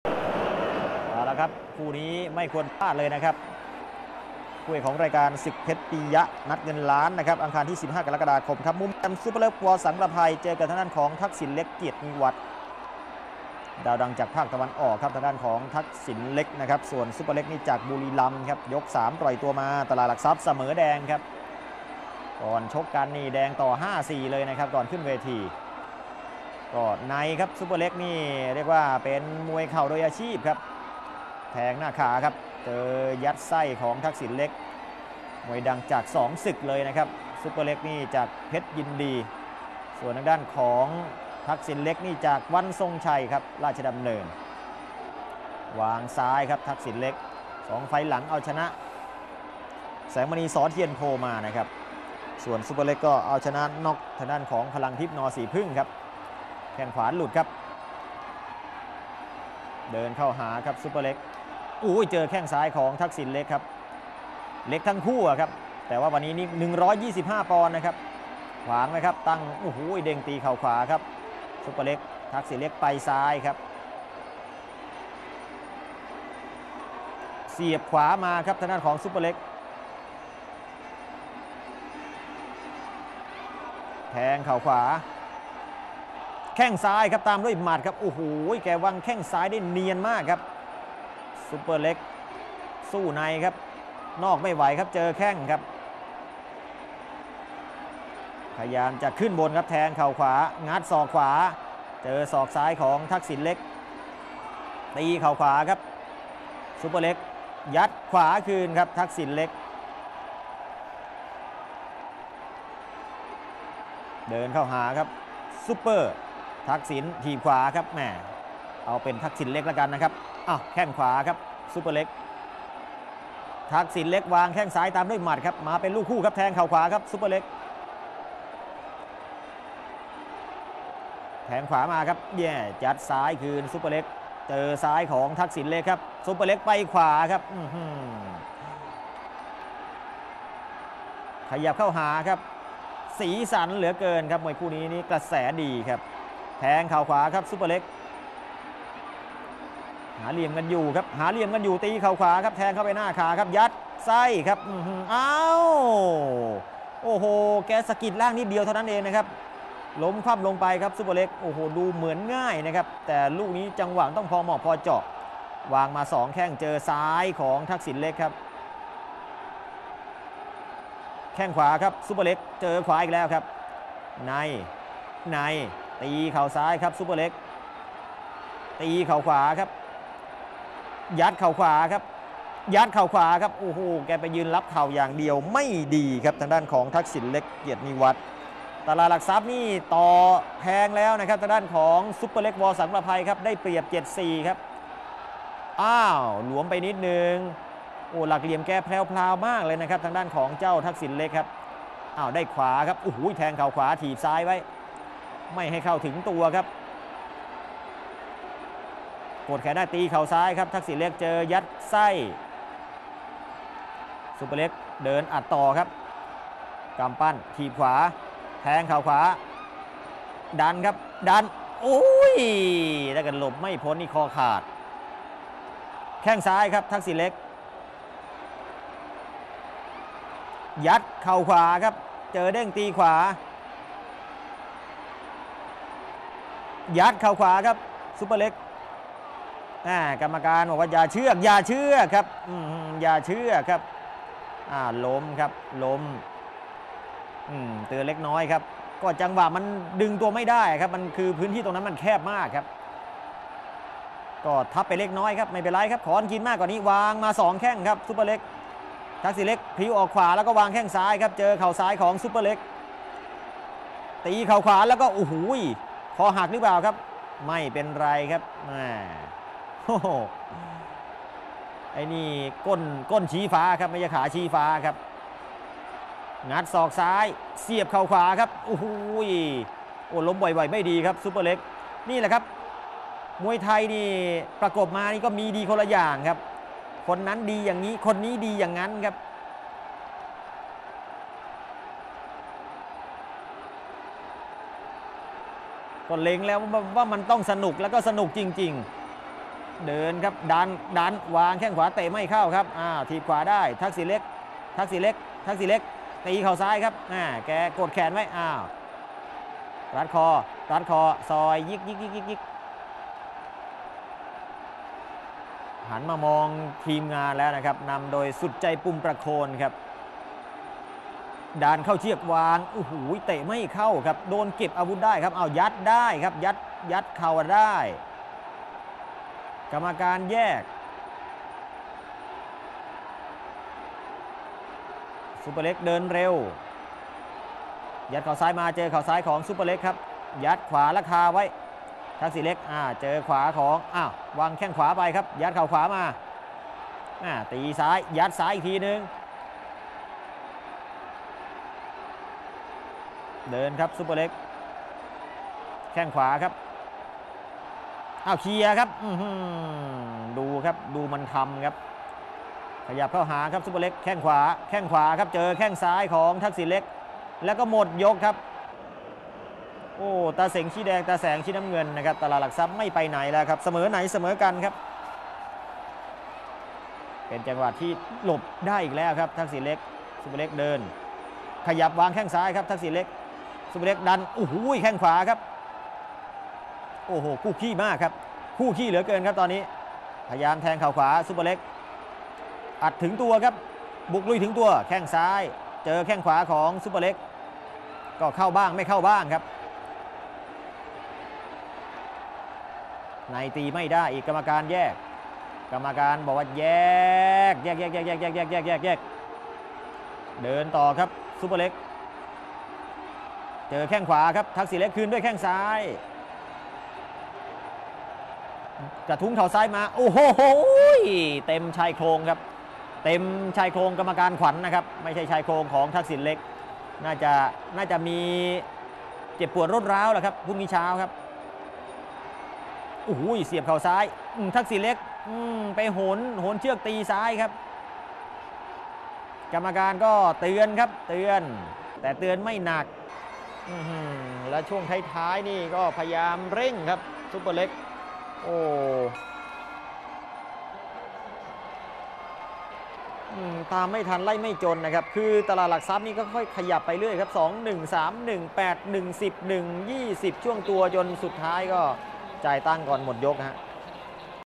Superlek Wor Sangprapai vs Thaksinlek Kiatniwat 15th July 2014
Muay Thai fight - Superlek Wor Sangprapai (red) vs Thaksinlek Kiatniwat (blue) at 125lbs. From Lumpinee Stadium, Bangkok Thailand on the 15th July 2014.